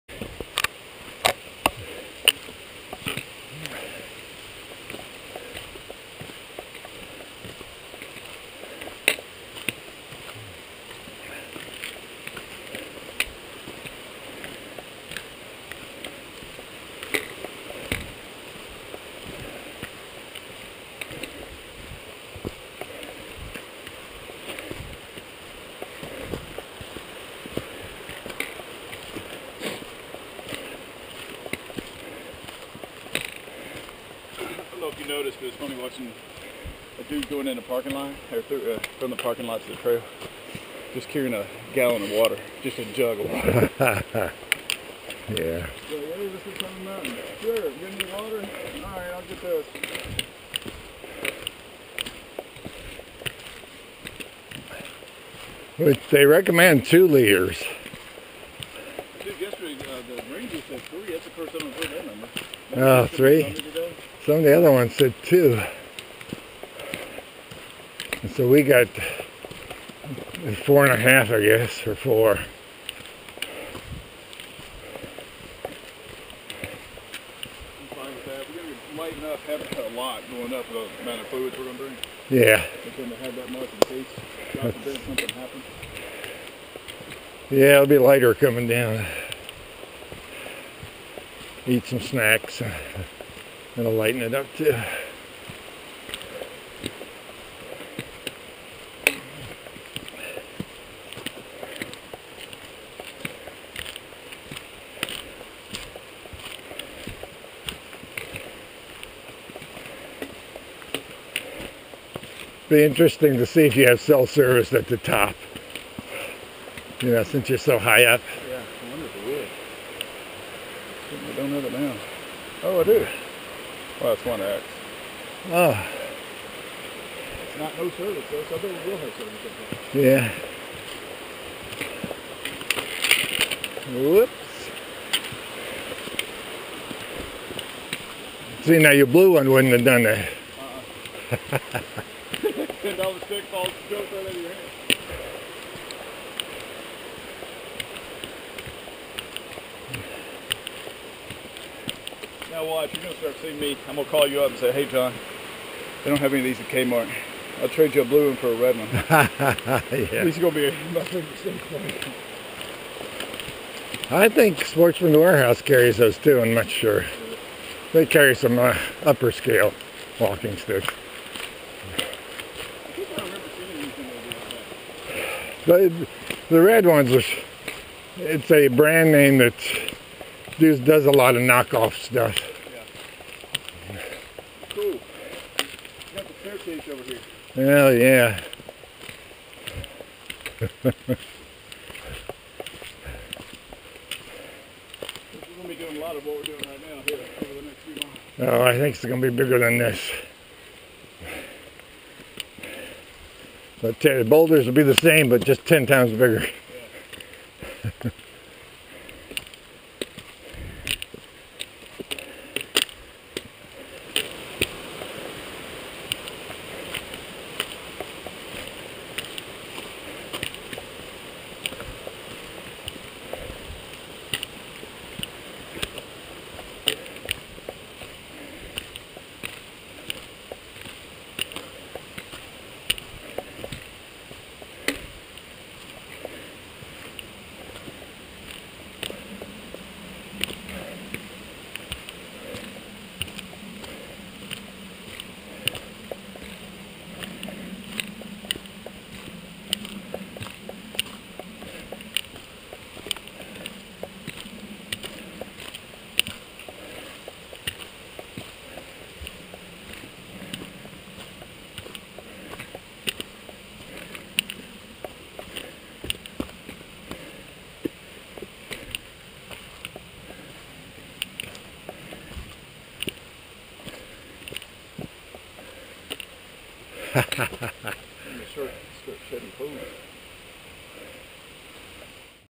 Mt.Katahdin (2 of 10)
Hiking toward Katahdin Stream Falls